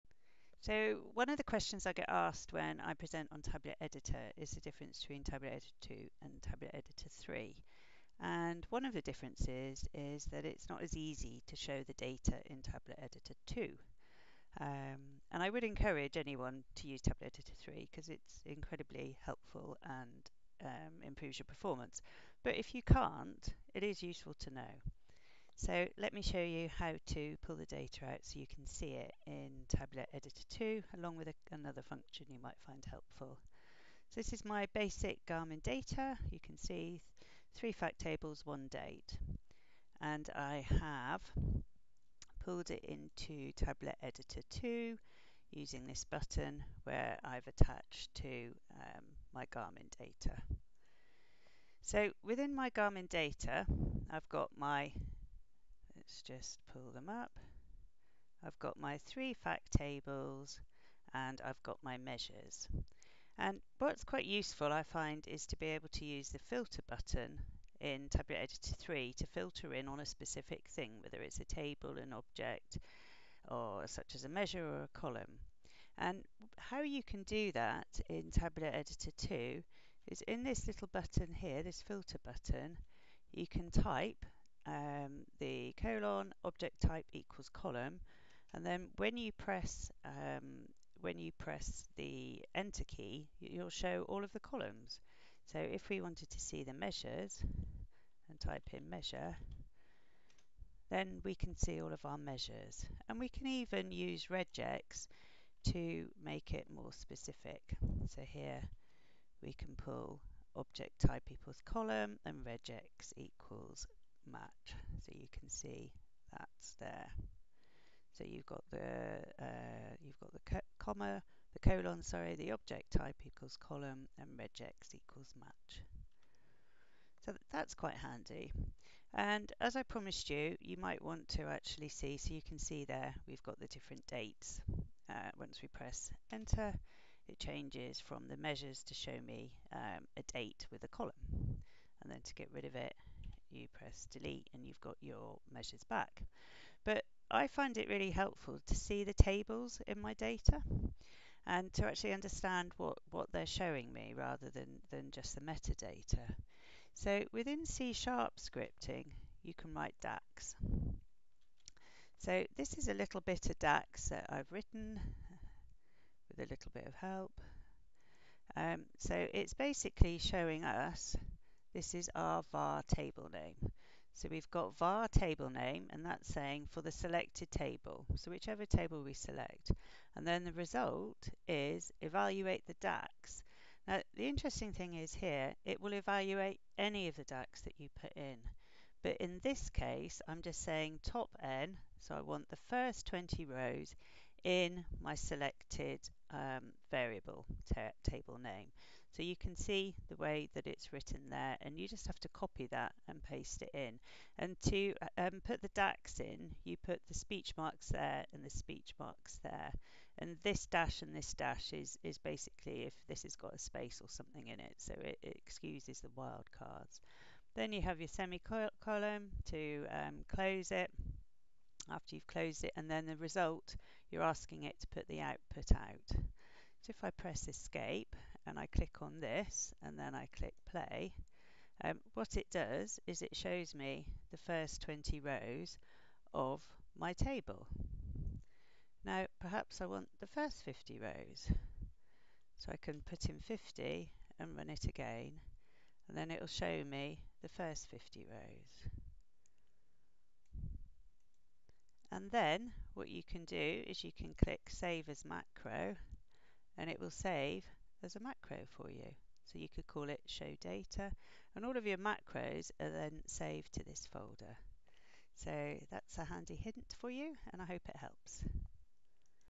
Tabular Editor 2 - Show Data and Filter
This video shows how to use C# scripting and DAX to show the data held within your tables.  Also included is how to filter your model by any object which together with RegEx gives you full filter functionality.